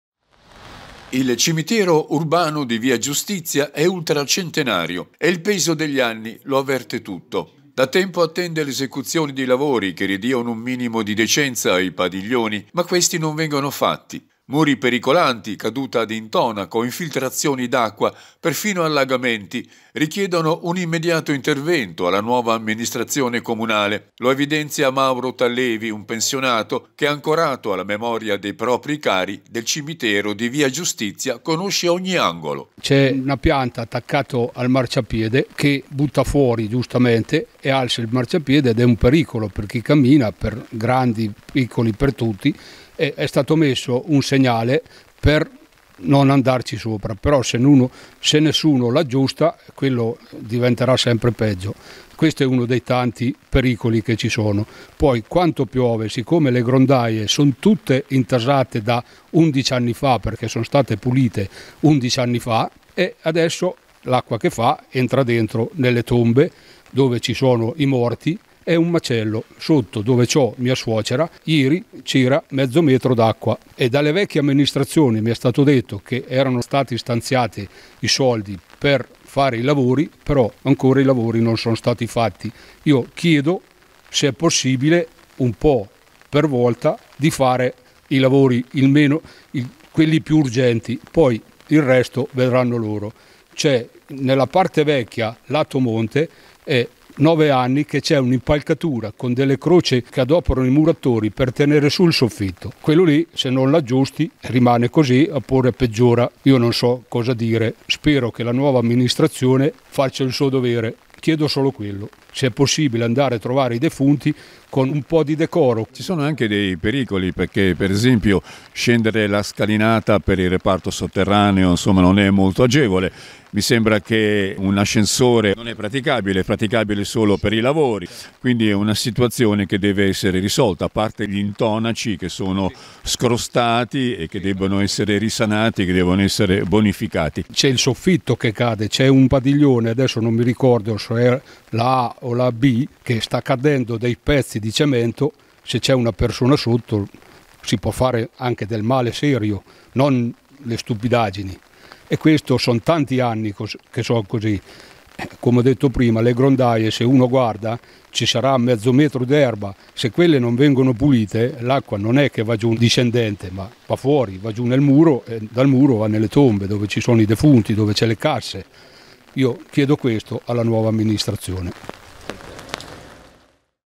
Cimitero urbano a fano, “non è un luogo dignitoso”
La pioggia di questi giorni ha infierito anche sul cimitero urbano di via della Giustizia, aggravando i problemi che esistono da tempo. “Non è un luogo dignitoso – ha dichiarato un cittadino – in cui custodire la memoria dei nostri cari defunti”.